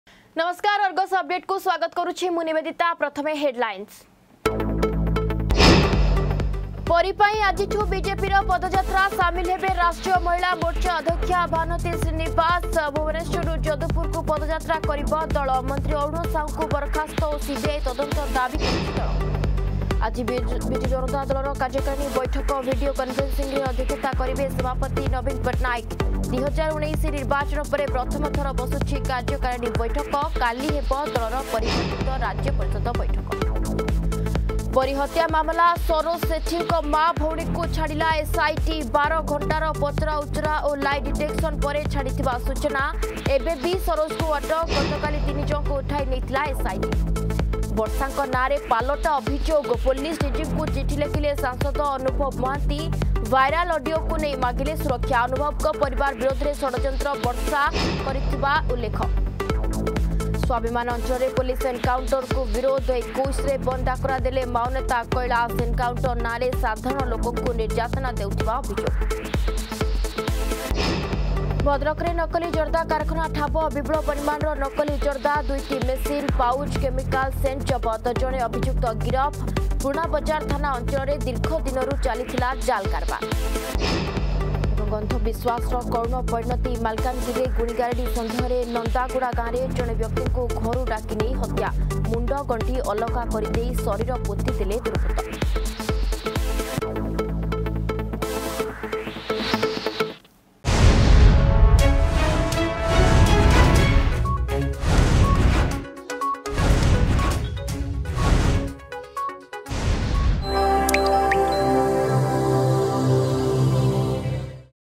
10AM HEADLINES
ପରୀ ପାଇଁ ଆଜିଠୁ ବିଜେପିର ପଦଯାତ୍ରା…
ଆଜି ବିଜୁ ଜନତା ଦଳର କାର୍ଯ୍ୟକାରିଣୀ ବୈଠକ…
ପରୀ ହତ୍ୟା ମାମଲା...
ବର୍ଷାଙ୍କ ନାଁରେ ପାଲଟା ଅଭିଯୋଗ... ପୋଲିସ
ସ୍ୱାଭିମାନ ଅଂଚଳରେ ପୋଲିସ ଏନକୋଉଣ୍ଟରକୁ ବିରୋଧ...
ଭଦ୍ରକରେ ନକଲି ଜର୍ଦ୍ଦା କାରଖାନା ଠାବ…
ଅନ୍ଧ ବିଶ୍ୱାସର କରୁଣ ପରିଣତି...